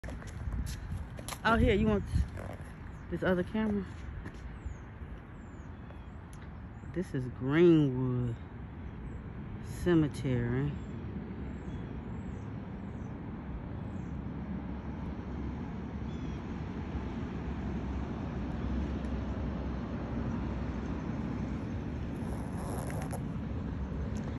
Greenwood North Cemetery
Greenwood Cemetery North, located in Goodlettsville, Tennessee, is a historic cemetery established in the late 1800s. It serves as the final resting place for many individuals and families from the Goodlettsville area and surrounding communities. The cemetery is known for its serene setting and contains graves dating back over a century, providing a glimpse into the area's history. Like many cemeteries, Greenwood Cemetery North holds significance for the local community, preserving the memories and legacies of those who have passed away.